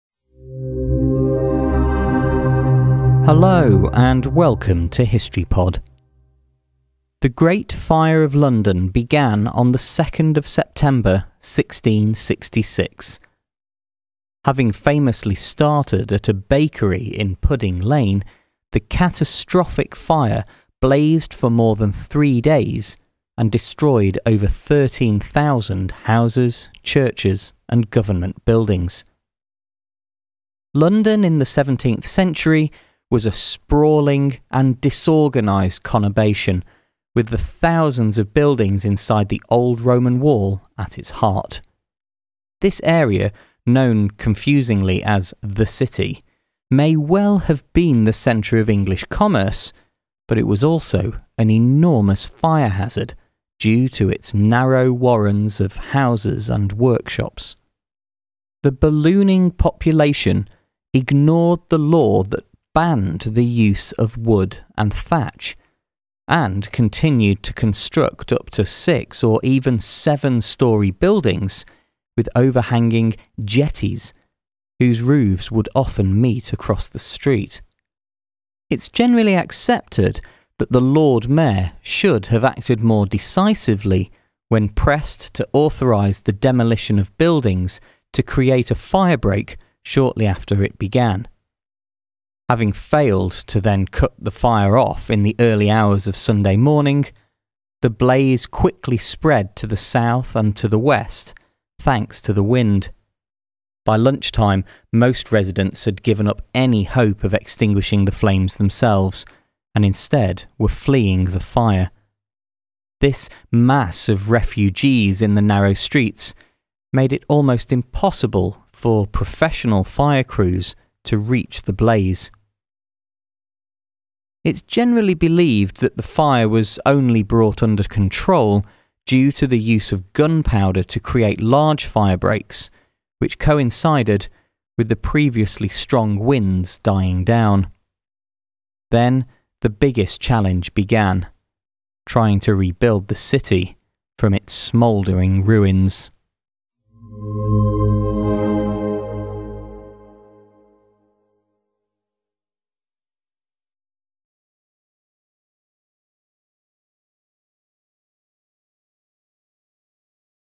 2nd September 1666: Great Fire of London breaks out in Pudding Lane
Having famously started at a bakery in Pudding Lane, the catastrophic fire blazed for more than three days and destroyed over 13,000 houses, churches and government buildings.

London in the 17th Century was a sprawling and disorganised conurbation, with the thousands of buildings inside the old Roman wall at its heart. This area – known confusingly as ‘the City’ – may well have been the centre of English commerce but was also an enormous fire hazard due to its narrow warrens of houses and workshops. The ballooning population ignored the law that banned the use of wood and thatch, and continued to construct up to six- or seven-storey buildings with over-hanging ‘jetties’ whose roofs would often meet.

It’s generally accepted that the Lord Mayor should have acted more decisively when pressed to authorise the demolition of buildings to create a fire-break. Having failed to cut off the fire in the early hours of Sunday morning, it quickly spread to the south and west thanks to the wind. By lunchtime most residents had given up any hope of extinguishing the flames themselves, and instead were fleeing the fire. This mass of refugees in the narrow streets made it almost impossible for professional fire crews to reach the blaze.

It’s generally believed that the fire was only brought under control due to the use of gunpowder to create large firebreaks, which coincided with the previously strong winds dying down. Then the biggest challenge began: trying to rebuild the city from the smouldering ruins.